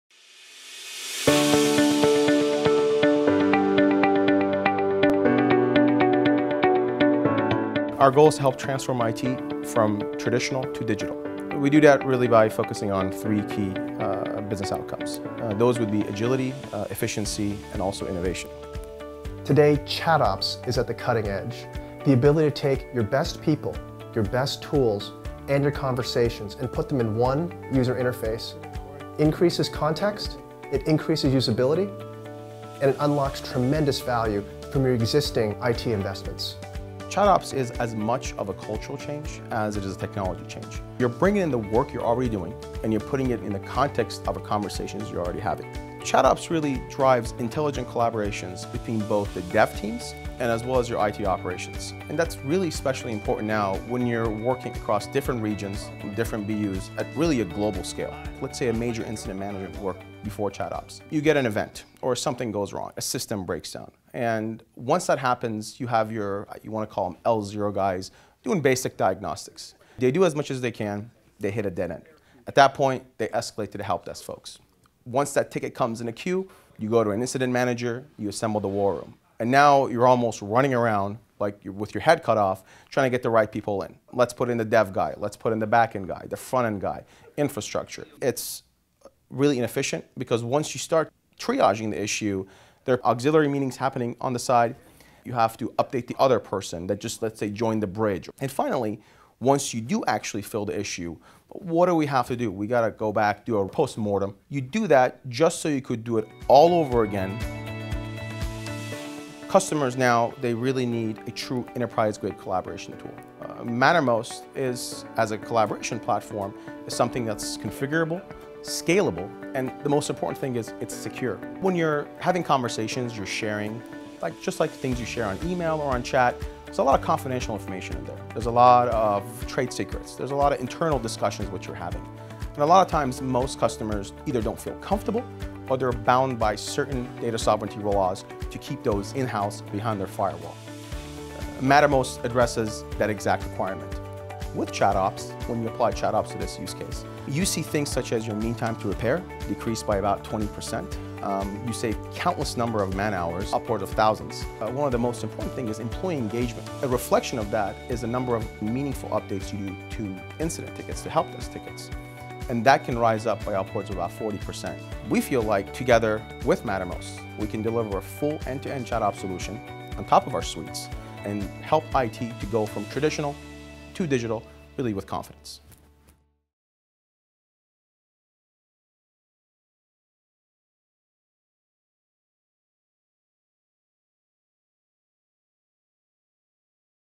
HPE and Mattermost offer private cloud Enterprise DevOps / ChatOps solutions
ChatOps is a vital component of the DevOps movement transforming IT organizations around the world. Enterprises that haven’t yet adopted ChatOps miss out on the significant gains in agility, efficiency, and innovation enjoyed by their smaller, faster-moving competitors.

To make the transition as seamless as possible, HPE and Mattermost have partnered to deliver a turnkey "Enterprise ChatOps" solution that increases collaboration and empowers organizations to respond more rapidly to crises while enhancing security and ensuring compliance.

Mattermost is the leading enterprise-grade, private cloud alternative to Slack, Yammer, and Microsoft Teams. It is used by thousands of organizations around the world for workplace messaging and is available in over a dozen languages.